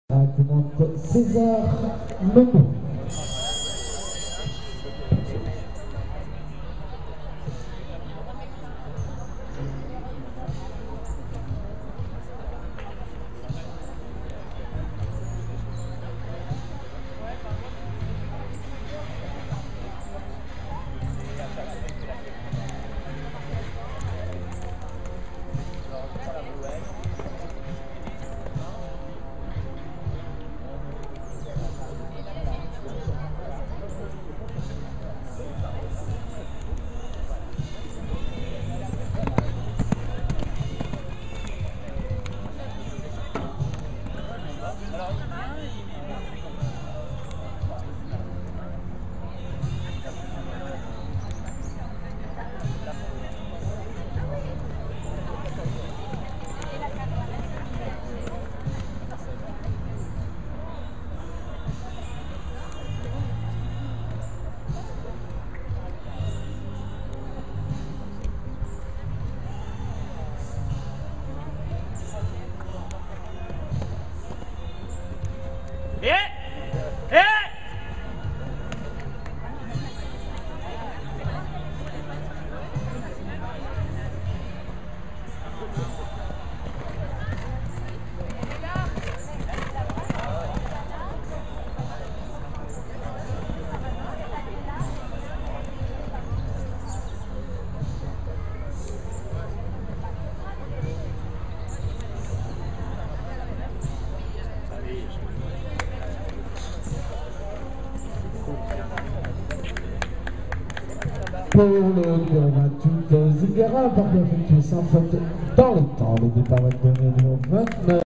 César et Zingara, préparatoire 1,00m à Neyron
César et Zingara, parcours sans faute : préparatoire 1,00m à Neyron le 2 octobre 2011.